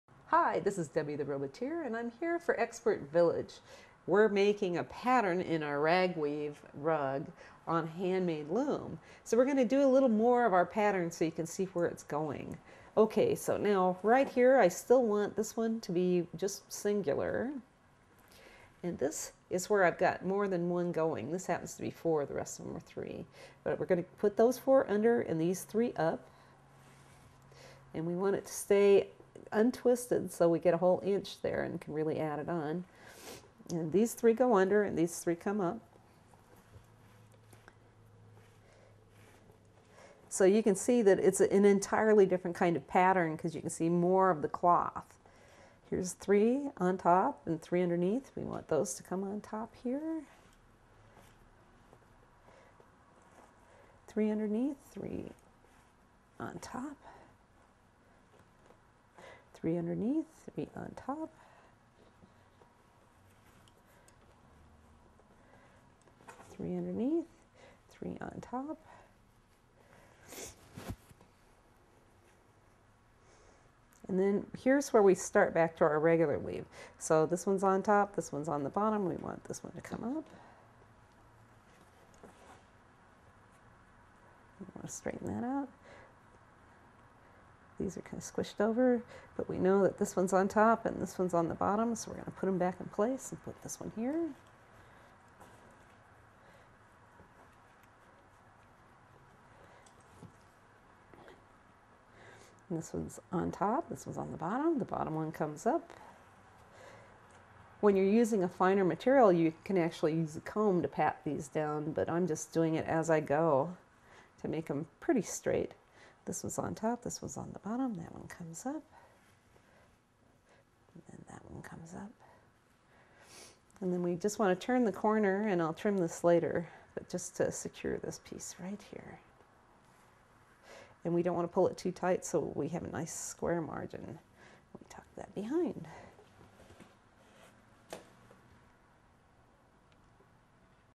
How to Weave on a Frame Loom : How to Weave a Rug Pattern on a Frame Loom
Weaving patterns into a rug on a frame loom. Learn about rug weaving and how to weave with a frame loom in this free video.

Expert: Deb Huglin Vogel
Bio: Deb Huglin Vogel is a professional sculptor in Iowa. She has been sculpting since she was a little girl. She was taught by Sandy Caldwell.
Filmmaker: Louis Nathan